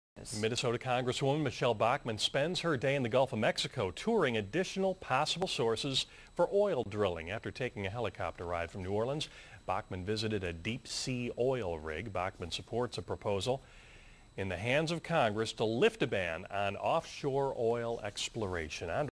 Touring an Oil Rig in the Gulf of Mexico
After an energy debate between House Democrats and Republicans at Tulane University in New Orleans, I had the chance to visit an oil rig in the Gulf. Here's a short little news clips about the experience.